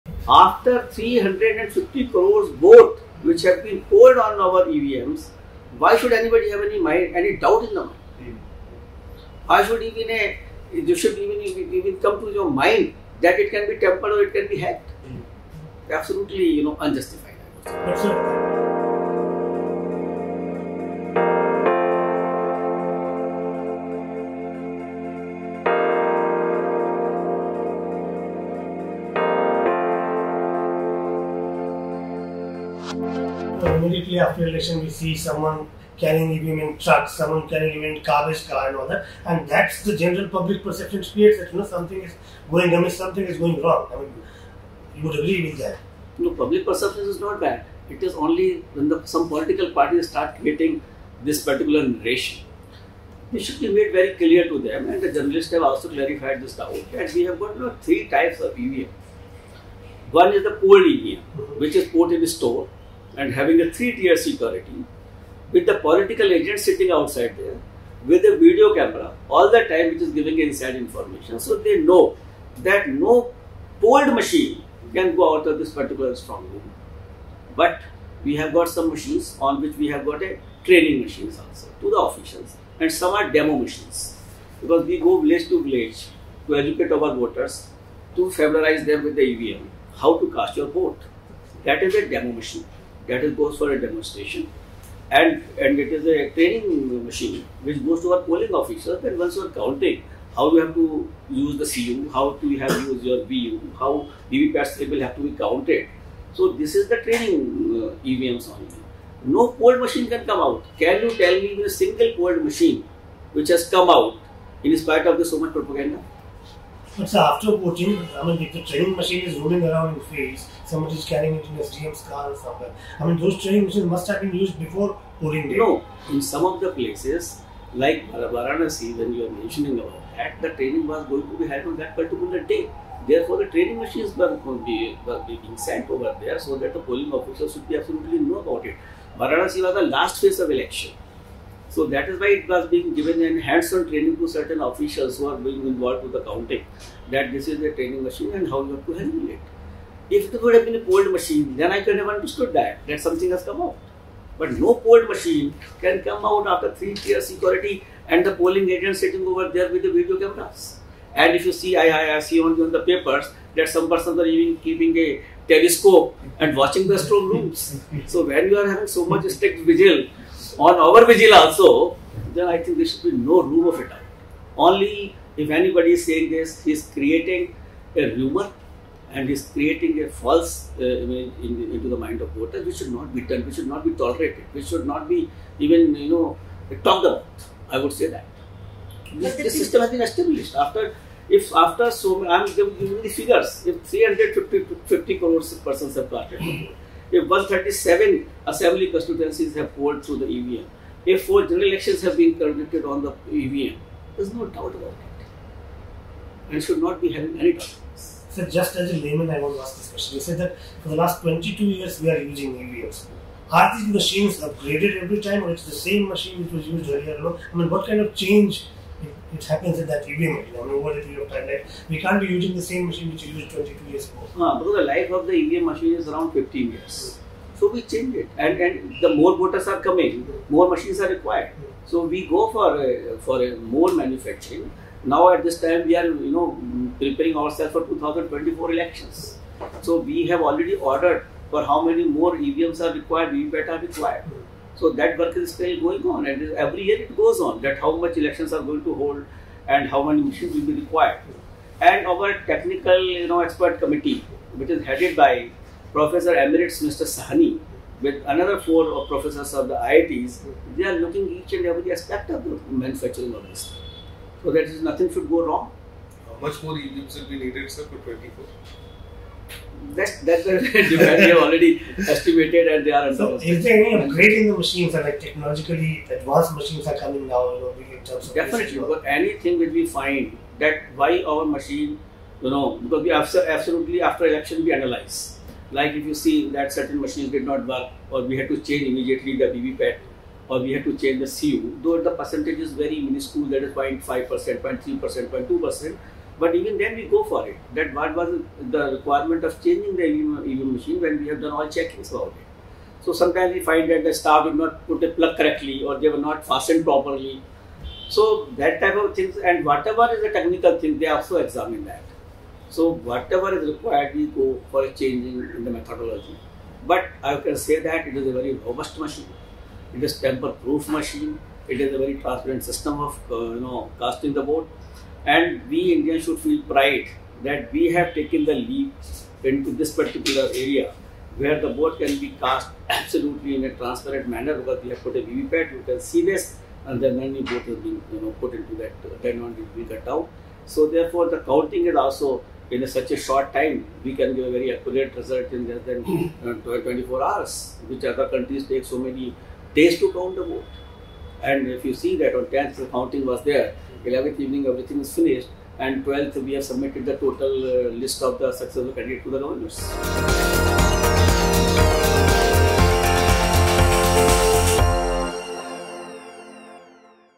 Watch: CEC Sushil Chandra on why polls conducted with EVMs are trustworthy
Political parties have always been vocal about the alleged tampering of electronic voting machines (EVMs). Chief Election Commissioner Sushil Chandra opens up about the allegations and the procedures undertaken by election commission to ensure the security of EVMs. Watch!